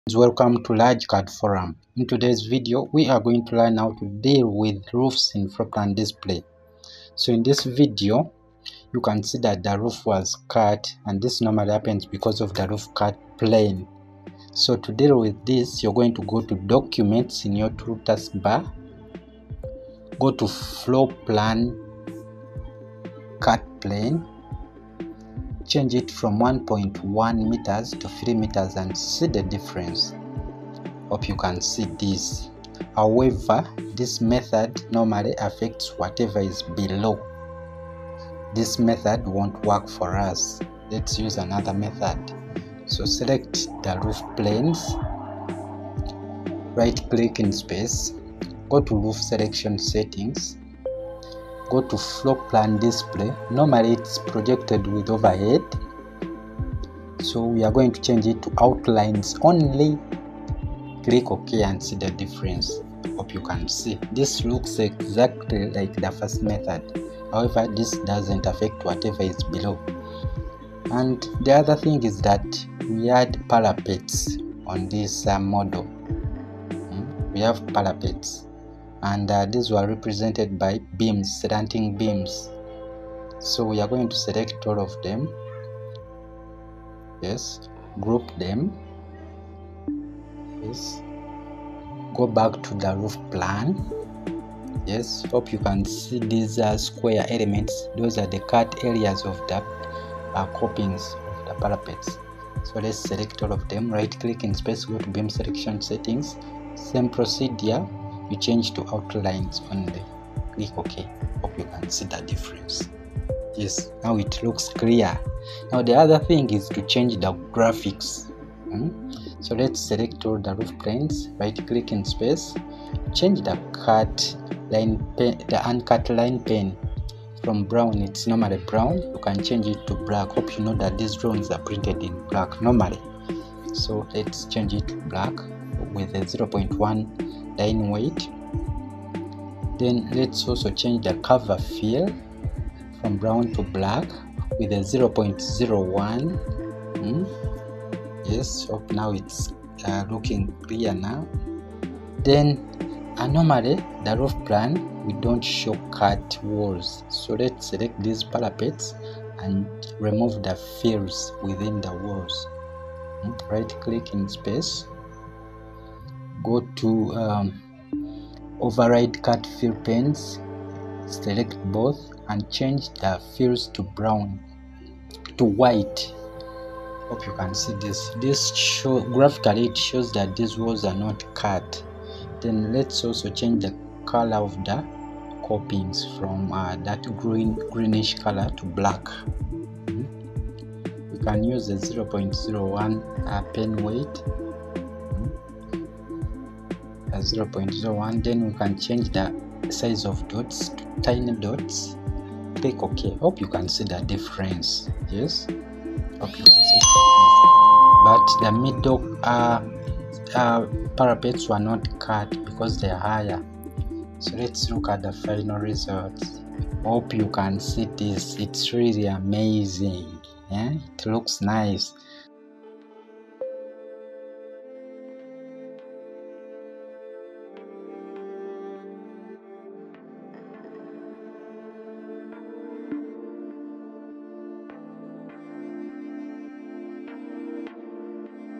Proper ROOF PLAN DISPLAY in ArchiCAD
# ARCHICAD ROOF PLAN DISPLAY EDITING archicad architecture design.
This tutorial clearly elaborated how to edit the roof plan using both the floor plan cut plane and the roof plan display settings.